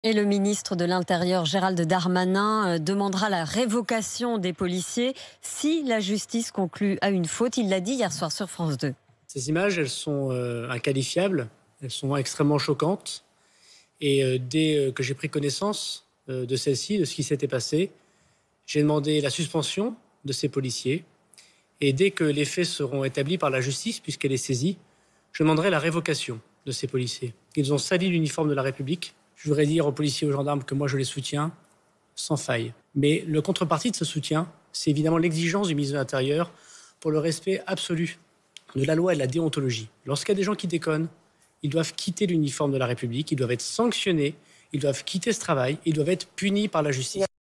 Les policiers soupçonnés d’avoir agressé le producteur de musique pourraient être révoqués
Producteur de musique agressé : le ministre de l'Intérieur Gérald Darmanin demandera "la révocation des policiers » si la justice conclut à une faute